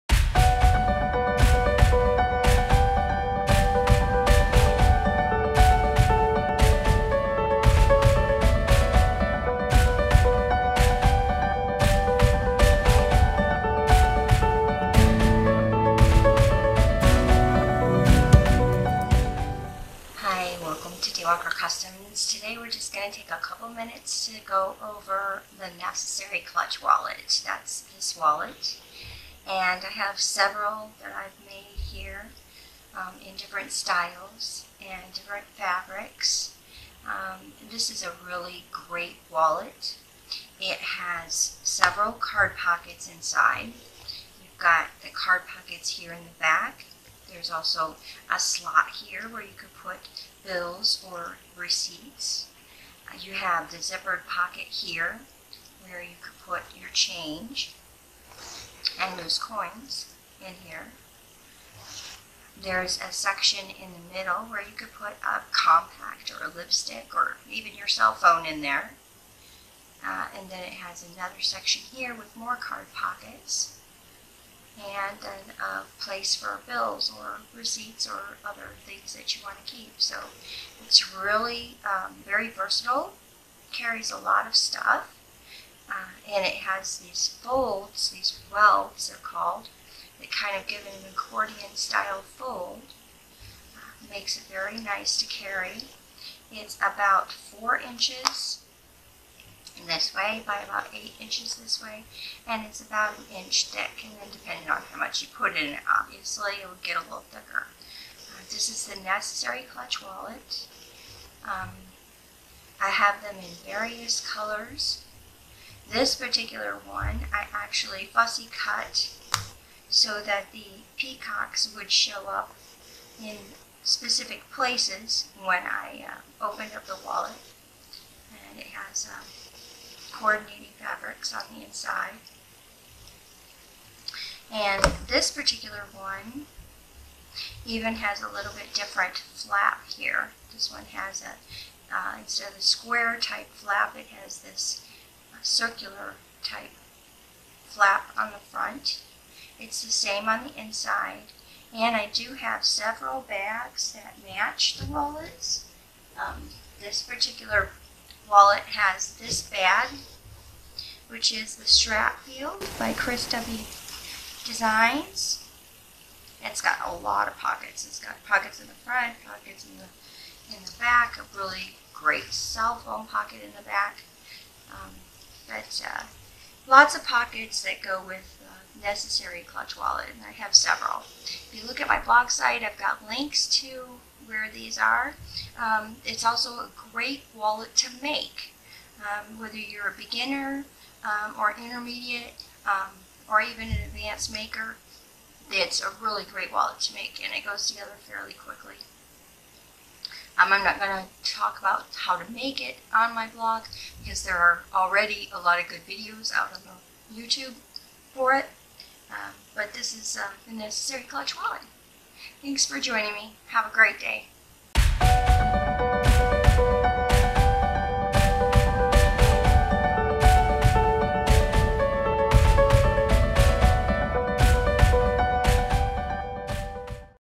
Necessary Clutch Wallet
This is an overview of this fabulous wallet.  The pattern is by Emmaline's Bags and has two sections of card pockets, a zipper pocket, and other slots/pockets inside.  for more on this great wallet.